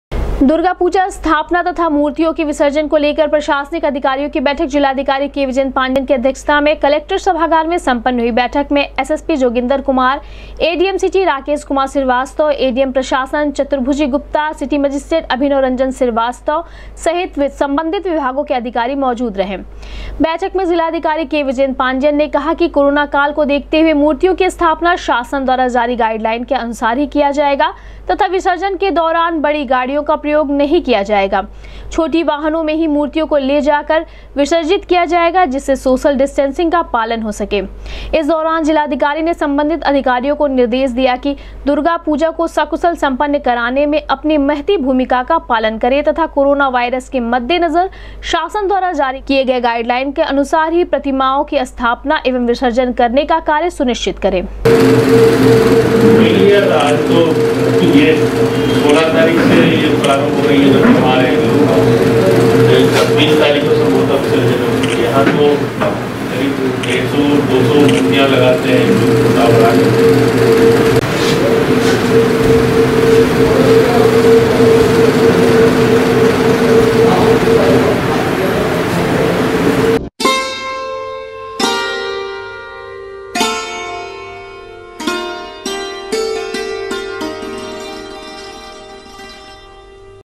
जिलाधिकारी की अध्यक्षता में सम्पन्न हुई दुर्गा पूजा स्थापना तथा मूर्तियों के विसर्जन को लेकर प्रशासन
जिलाधिकारी की अध्यक्षता में सम्पन्न हुई दुर्गा पूजा स्थापना तथा मूर्तियों के विसर्जन को लेकर प्रशासनिक अधिकारियों के साथ बैठक,शासन के गाइडलाइन का कड़ाई से पालन करने के दिये निर्देश।।।